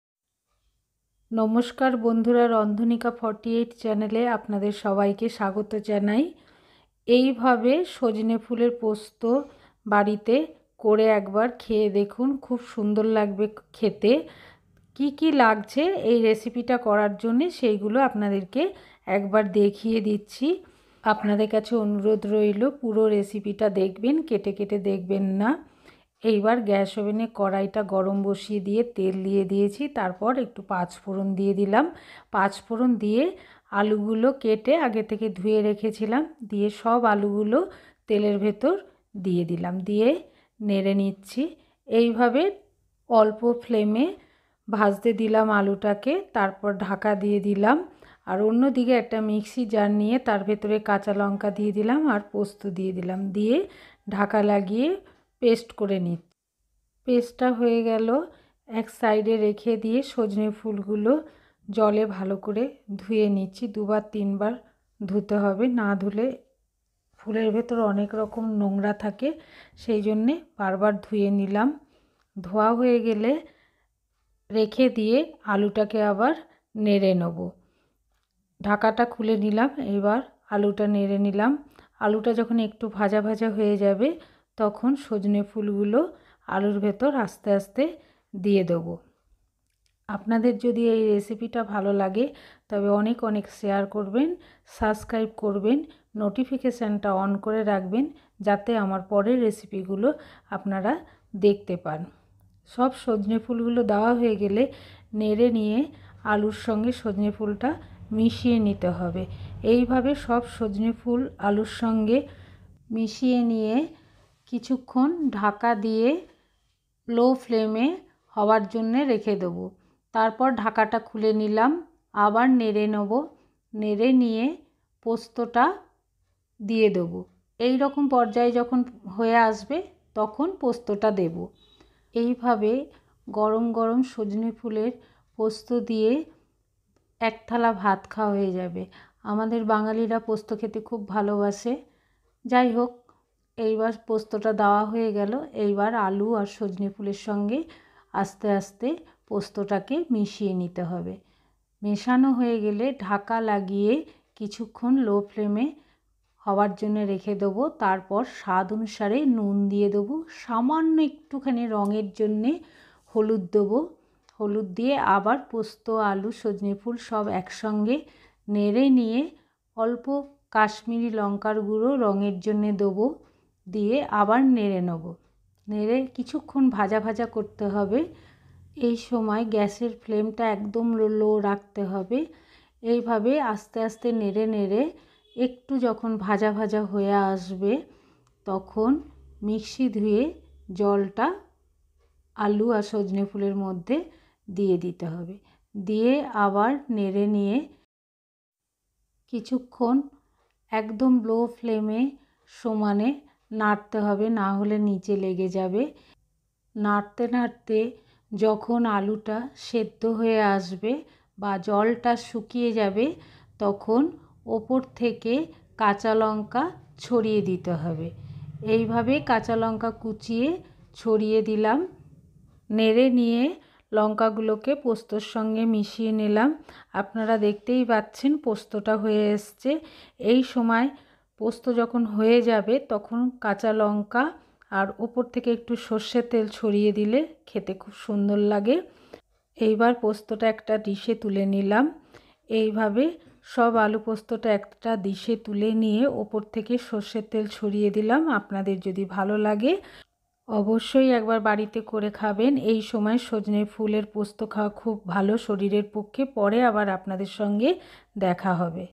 নিরামিষ সজনে ফুলের পোস্ত একবার এইভাবে করে দেখুন ll Sojne Phuler posto recipe
সজনে ফুলের পোস্ত ll Sajne phuler posto

উপকরণ:-
        সজনে ফুল, আলু, কাঁচা লঙ্কা, পাঁচফোরন, লবন, হলুদ, পোস্ত, তেল।

my facebook. Mousumi Mukherjee